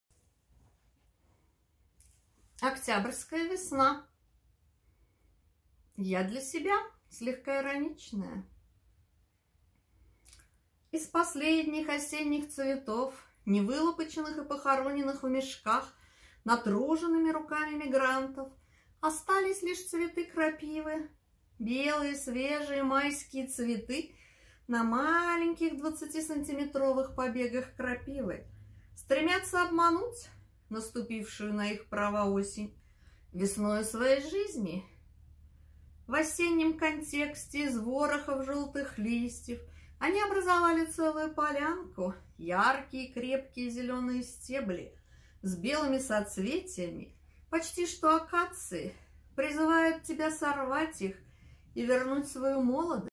Октябрьская весна
Стихи, верлибр
  Я-для-себя (слегка ироничное)
Из последних осенних цветов, не вылопаченных
И похороненных в мешках натруженными руками мигрантов,
Остались лишь цветы крапивы

Белые свежие майские цветы на маленьких
двадцатисантиметровых побегах крапивы
стремятся обмануть наступившую на их права осень
весною своей жизни

В осеннем контексте из ворохов желтых листьев
Они образовали целую полянку: яркие и крепкие зеленые стебли
С белыми соцветиями почти что акации призывают тебя сорвать их и вернуть свою молодость
Заплатив «всего лишь» обожженными саднящими ладошками

Не пробуй нарвать их в перчатках, ибо – помнишь старую сказку
О двенадцати заколдованных в лебедей братьях и одной сестре,
Которая вязала им рубашки из крапивы:
Только сорваннаяголойрукой крапива способна сотворить с тобой
Чудо весны